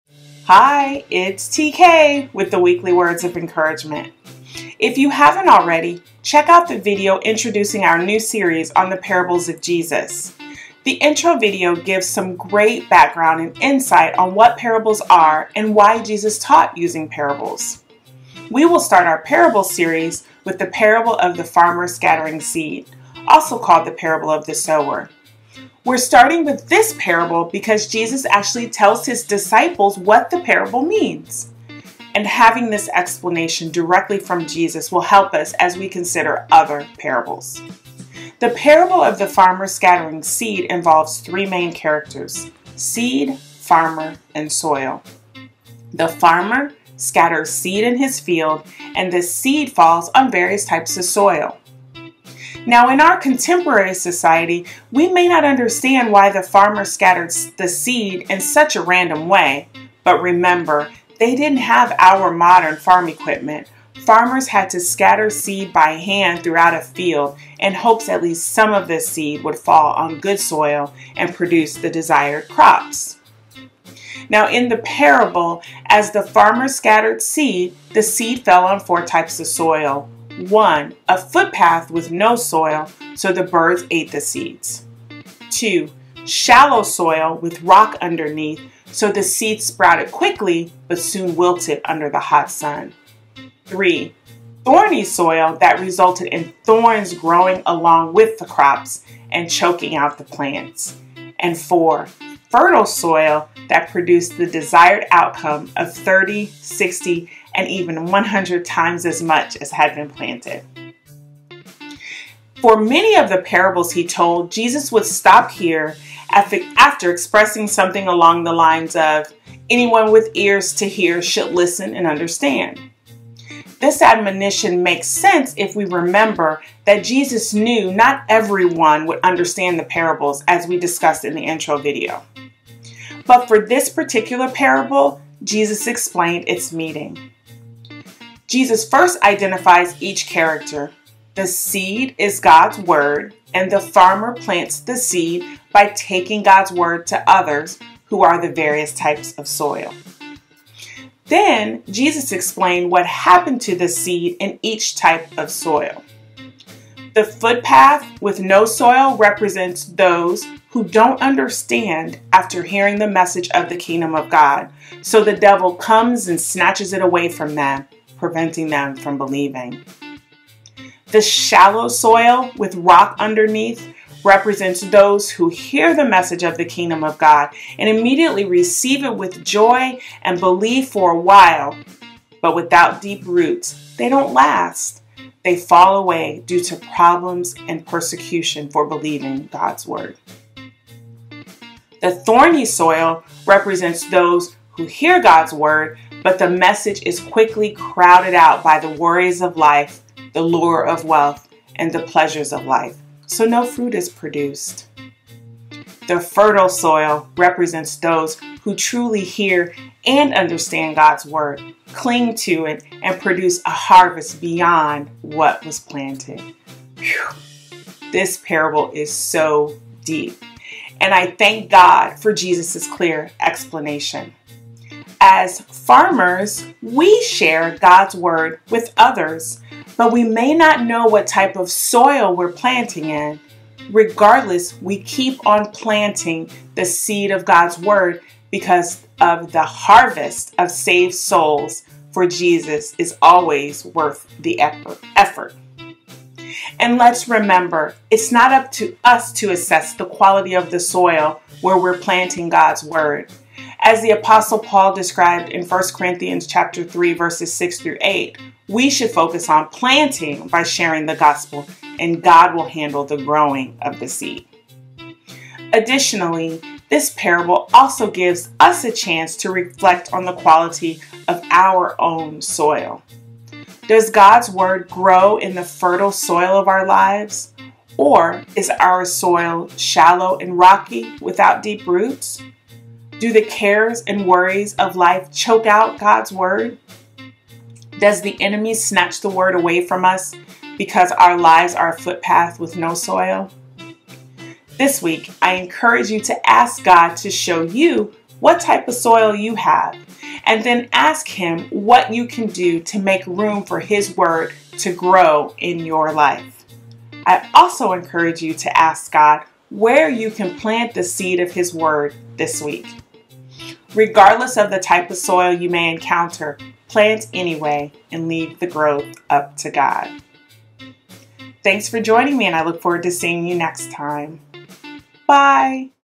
Planting God's Word (Parables of Jesus series) • Weekly Words of Encouragement
We will start our Parables of Jesus series with the Parable of the Farmer Scattering Seed (also called the Parable of the Sower).  This parable involves 3 main characters:  seed; farmer; and soil.

The farmer scatters seed in his field, and the seed falls on various types of soil.

The seed is God’s word and the farmer plants the seed by “taking God’s word to others,” who are the various types of soil.

We’re starting with this parable because Jesus actually tells His disciples what the parable means!  And having this explanation directly from Jesus will help us as we consider other parables in this series.